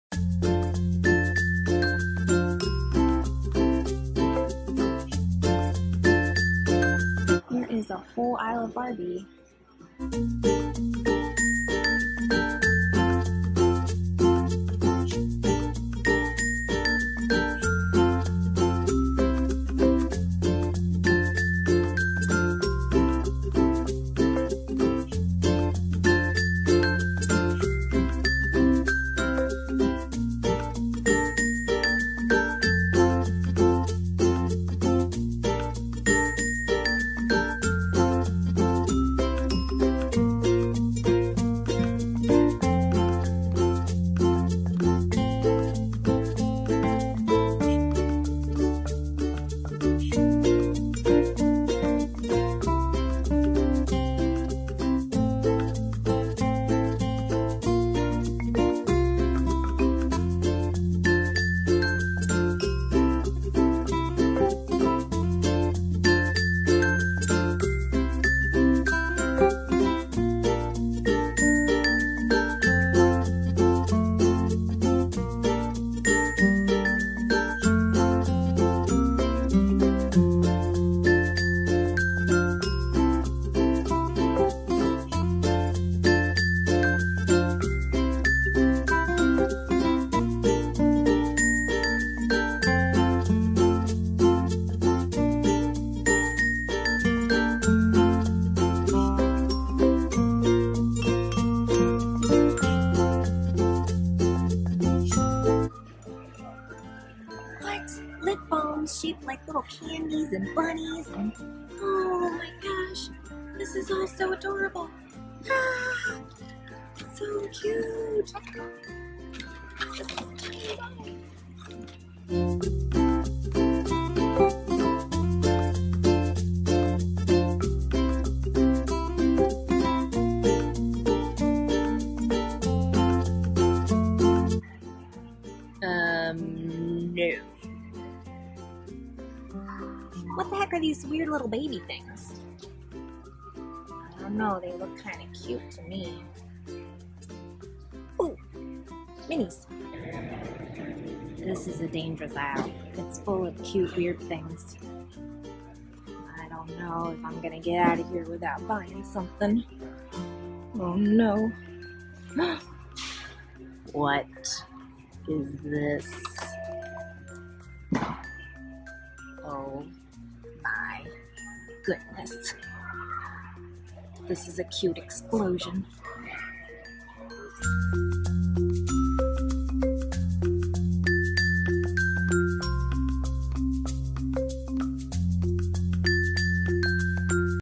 Doll shopping at ToysRUs!
I hadn't stepped into a physical toy store in about 10 years.